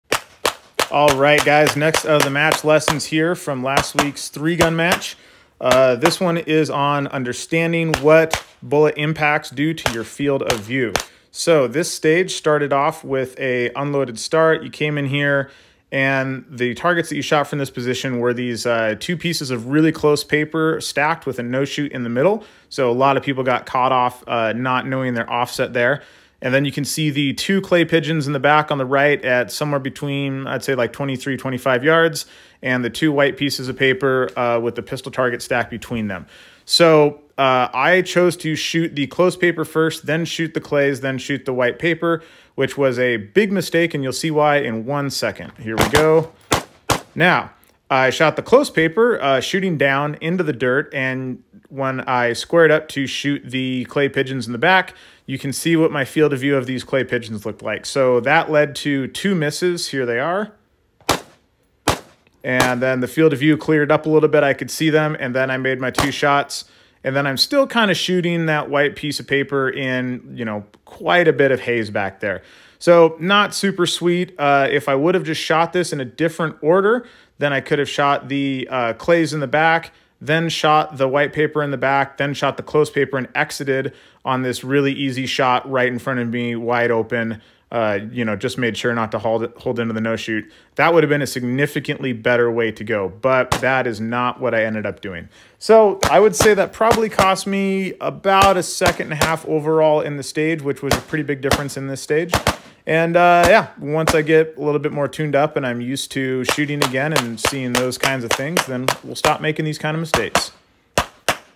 Match Lessons - Bullet Splash
The next of the lessons from last weekend's PCSL 3 Gun match is on understanding the visual impacts of bullet splash when shooting close targets. This is an example of me doing a poor job and living with the consequences.